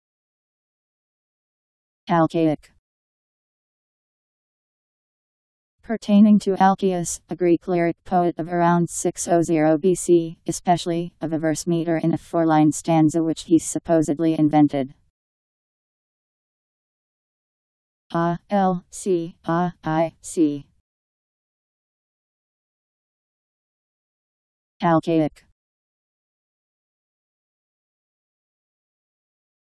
Alcaic Meaning
Video shows what alcaic means. pertaining to Alcaeus, a Greek lyric poet of around 600BC; especially, of a verse meter in a four-line stanza which he supposedly invented.  Alcaic Meaning. How to pronounce, definition audio dictionary. How to say alcaic. Powered by MaryTTS, Wiktionary